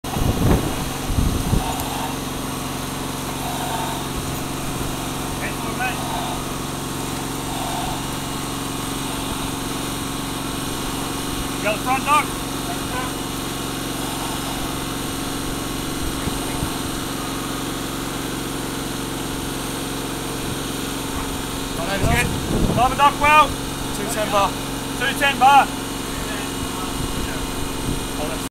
Sep 17 2010 - VID00130.AVI
Just completed a dive and hoags is attending me.